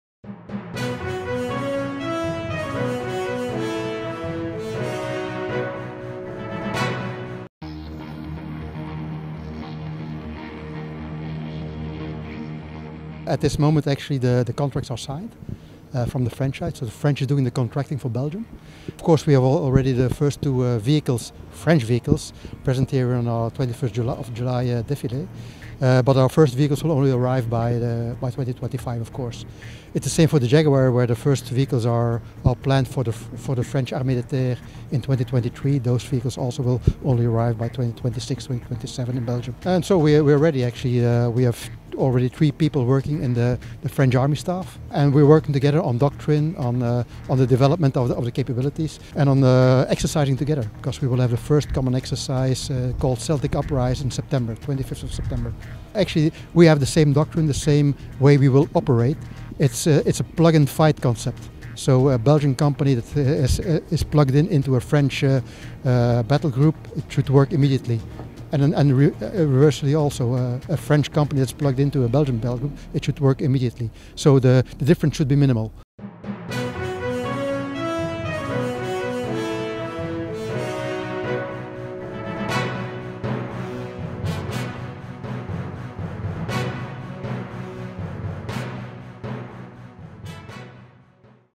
General Thys explains CaMo program new acquisition for Belgian army armored military equipment
Army Recognition editorial team interview with Major General Marc Thys, Belgian Army Commander who explains the CaMo (Capacité Motorisé - Motorized Capacity) program and future acquisition of Belgian army including military equipment and armored vehicles.